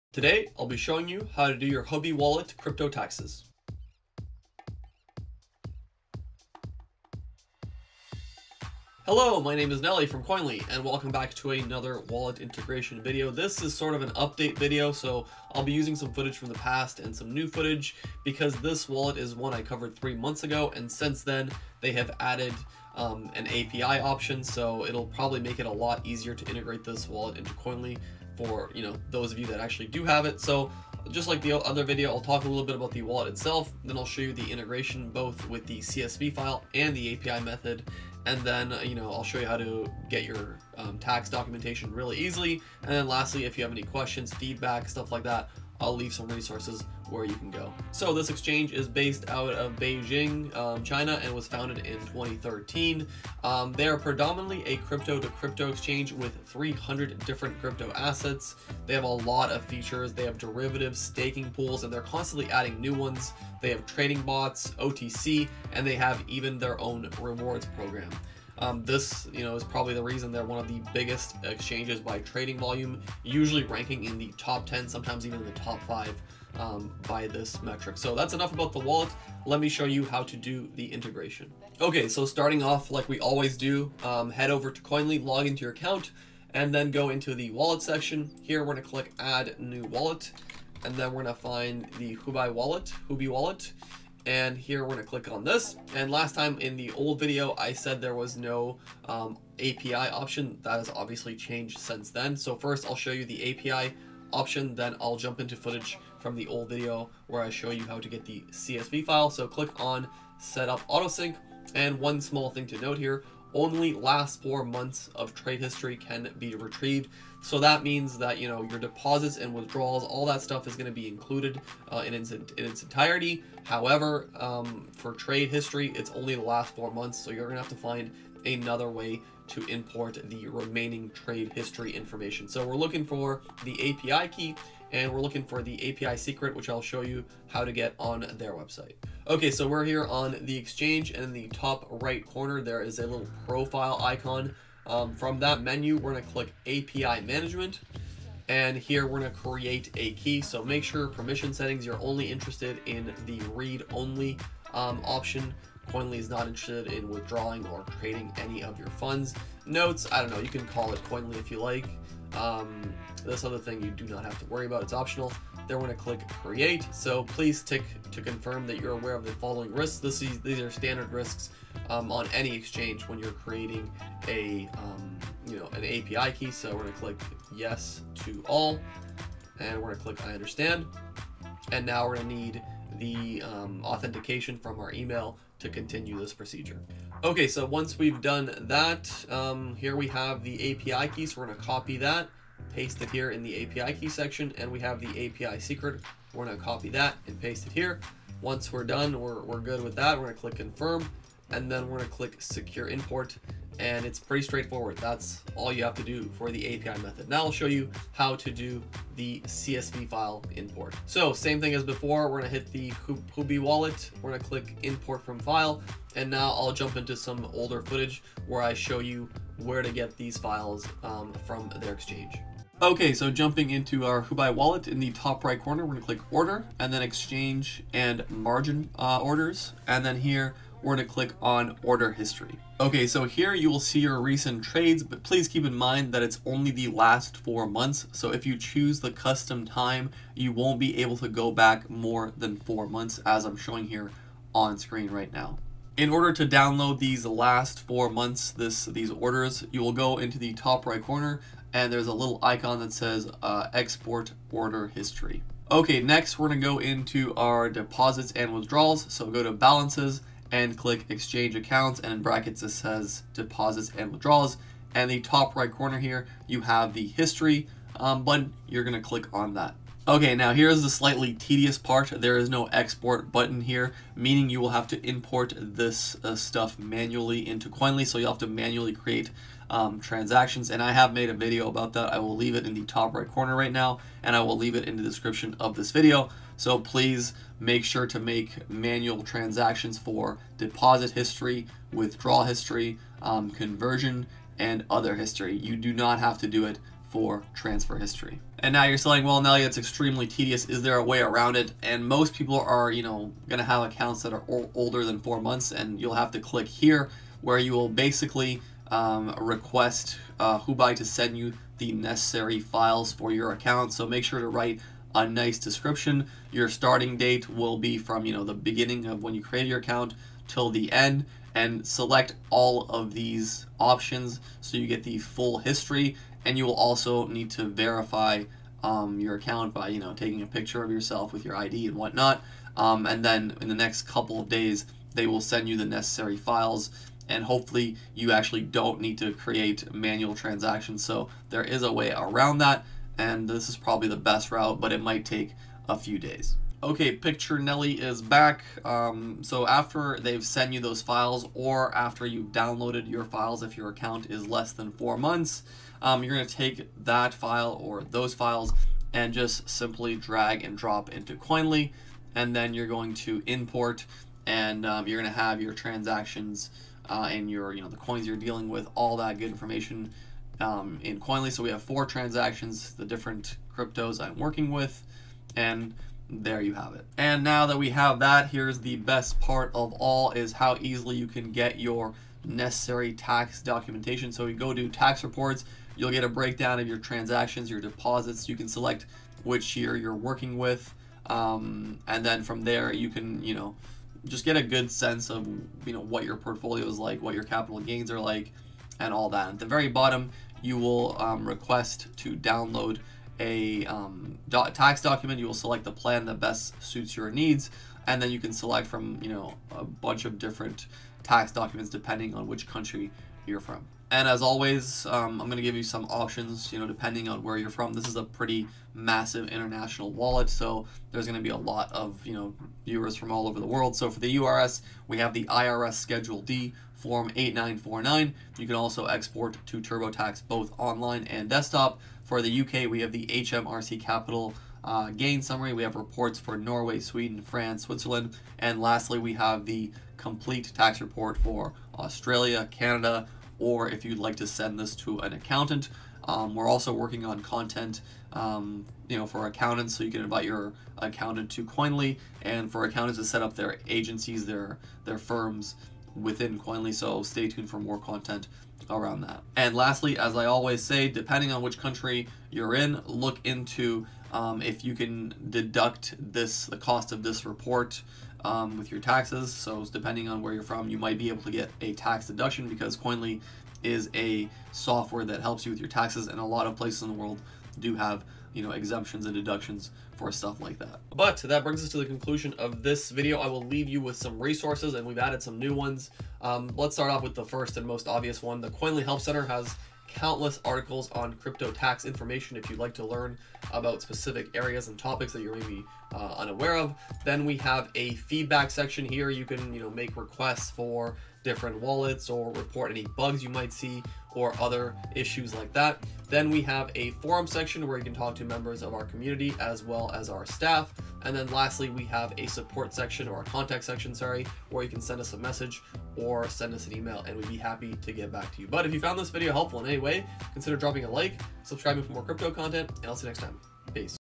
How To Do Your Huobi Crypto Tax FAST with Koinly - 2025
Trading with Huobi ? Not sure how to do your crypto tax? Worried about the IRS or your country’s tax authority? Relax, we've got you! Watch and learn with Koinly.

Using Huobi and wondering how to do your Huobi crypto taxes before the IRS, HMRC, CRA, or the ATO tax deadline? It’s really easy! Today I’ll show you have to quickly import your Huobi transactions into Koinly -  by importing different CSV files into Koinly. Koinly then calculates your cryptocurrency taxes and generates a fully compliant tax report that you file with yourself, or send to your crypto tax accountant. Let’s get your crypto tax sorted!

[00:00] Introduction
[00:50] Integration
[4:00] How To Download Tax Reports

Want more info? Check out our step-by-step guide on how to get your Huobi crypto tax done with Koinly